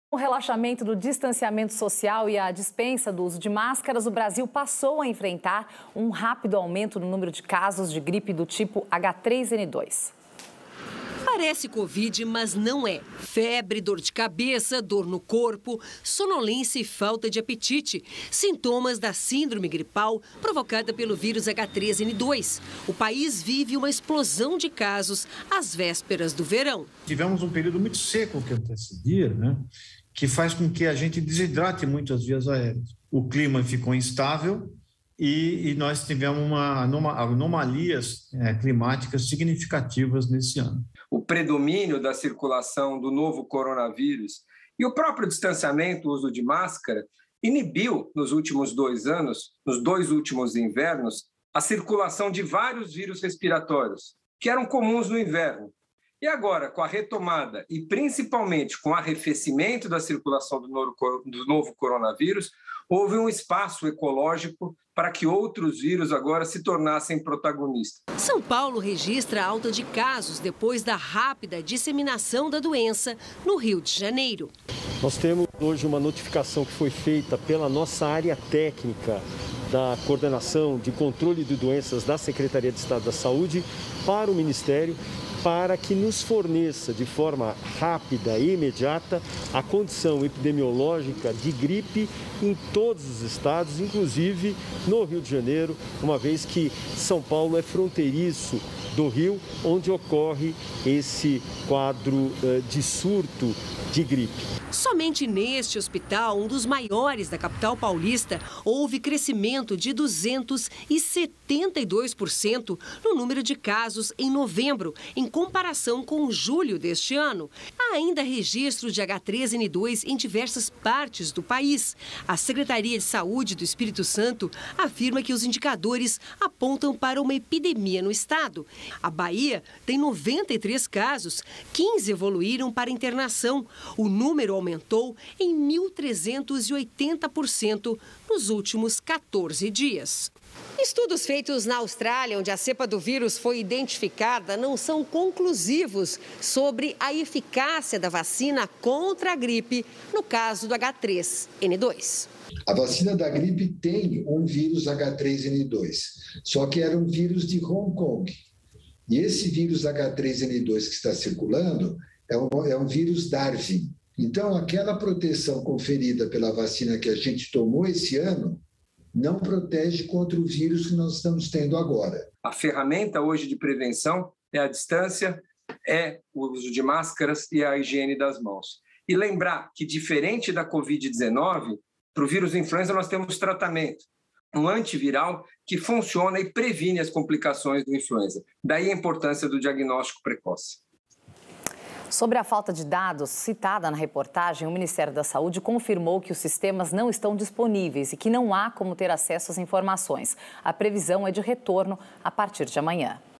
Gripe H3N2 se espalha pelo país
Surto acontece às vésperas do verão e possui sintomas parecidos com os da Covid-19. São Paulo e Rio de Janeiro sofrem epidemia da gripe.